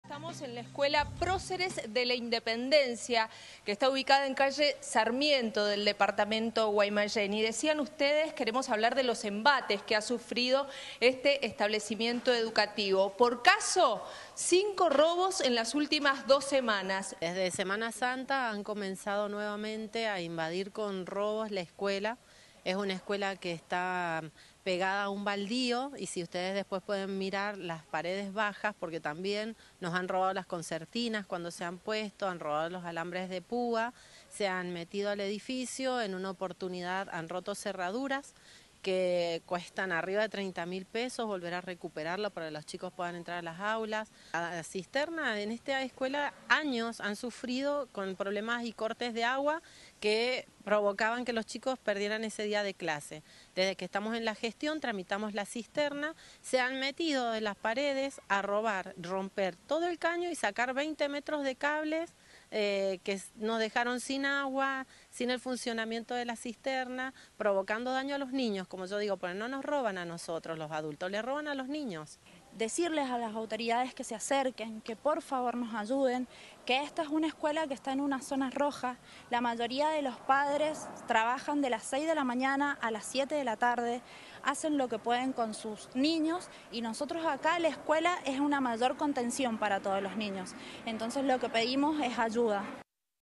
Más de 5 robos en un mes en una escuela de Guaymallén
La docente relata que la escuela fue asaltada más de 5 veces en un mes debido a que se encuentra al lado de un baldío. No solo los robos son el problema, el dañó causado en cerraduras también es una complicación.